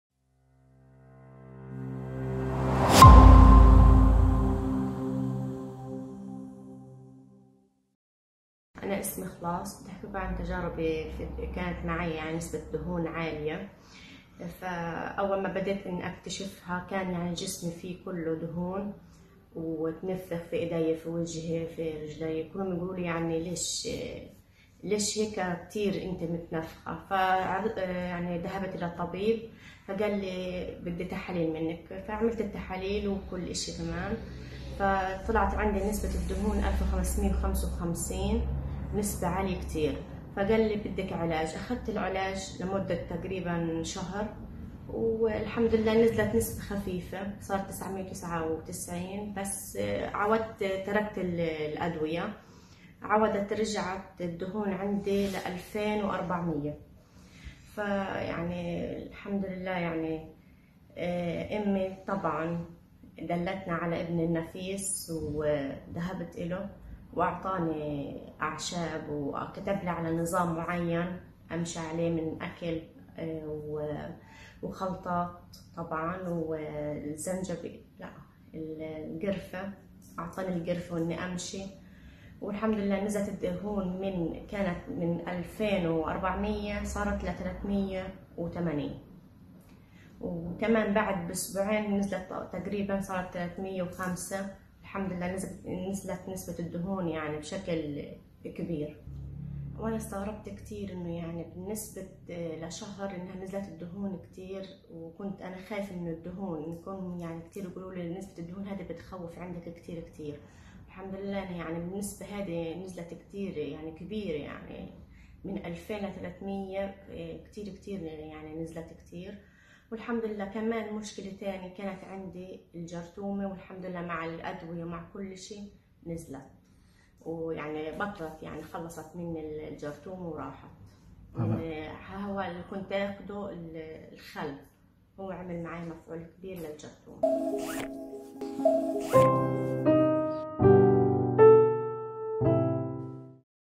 كيف تتخلص من ارتفاع الدهون الثلاثية خلال شهر "شاهد الوصف"
كيف تخلصت إخلاص من ارتفاع الدهون الثلاثية خلال شهر
من 2200 إلى 350 ... من خلال هذه الوصفة فقط

الوصفة:
- شرب قرفة عيدان مع كركم مرتين يومياً
- مشي نص ساعة يومياً
- تناول خل ابن النفيس ملعقة كبيرة على كاس ماء بعد الغداء
- سلطة مكونة من " ملفوف ابيض زهراء بيضاء جزرة بصلة كزبرة خضراء خيارة مع ملعقة خل وربع ملعقة صغيرة قرفة عيدان " بنعمل سلطة صحن ويؤكل"